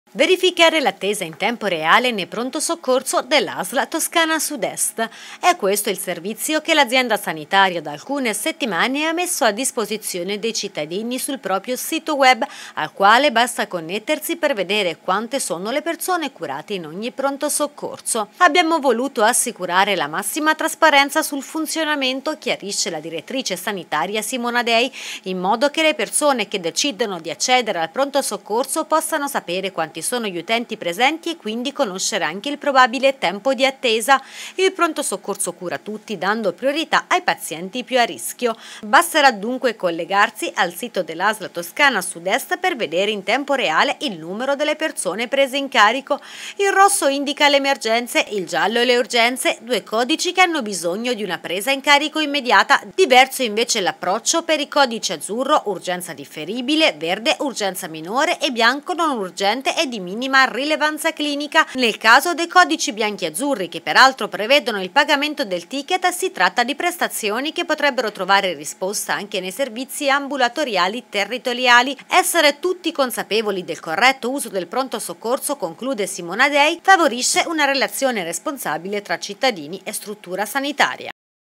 Pronto soccorso in tempo reale: online i pazienti presenti ed i casi gestiti
Verificare l'attesa in tempo reale nei pronto soccorso della Asl Toscana Sud est. È il servizio che l'azienda sanitaria, da alcune settimane, ha messo a disposizione dei cittadini sul proprio sito web al quale basta connettersi per vedere quante sono le persone curate in ogni Pronto Soccorso del territorio della asl Toscana sud est.